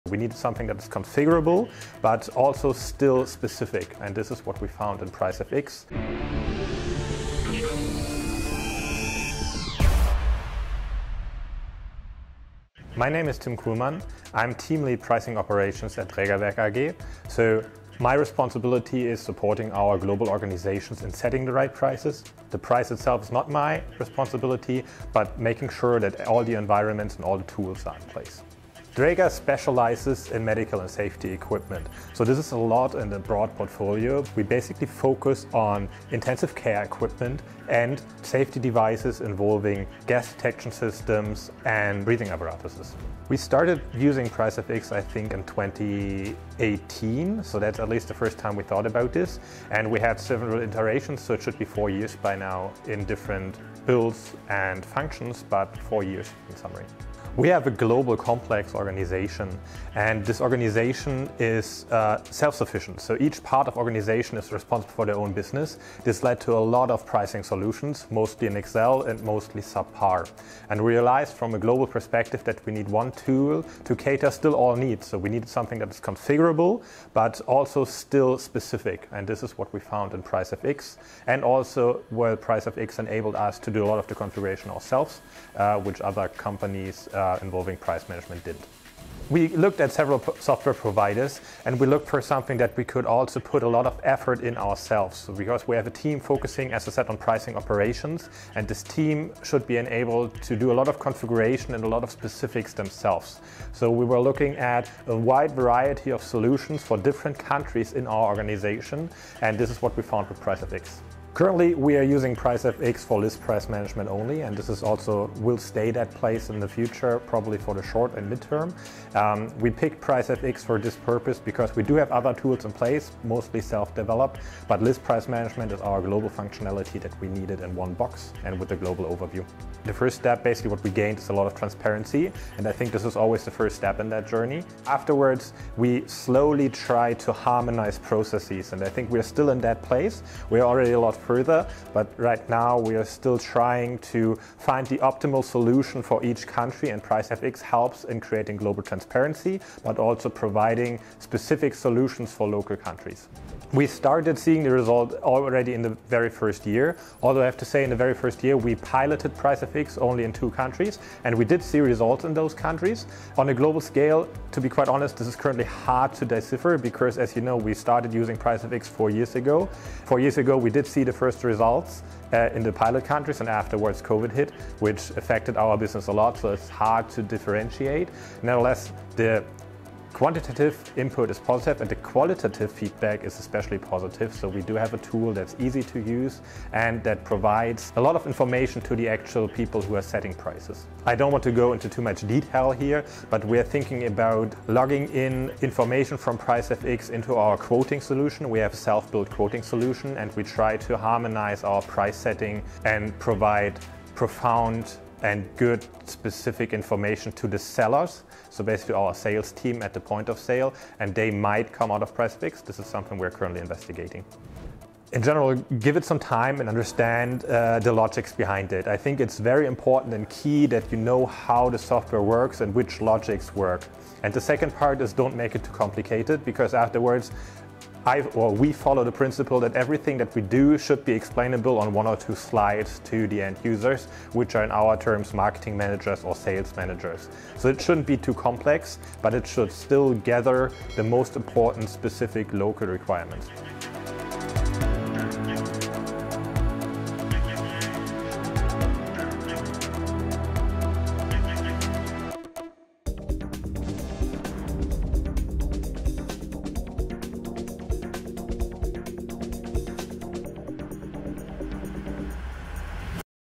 Draeger: A Pricing Success Story
Drager has been using Pricefx solutions for over 4 years! Find out why Tim Kuhlmann is a fan of Pricefx's List Management capabilities, the Pricefx team, and their plans for the future.

Want to start a conversation with us and see how we can help you to reach your goals?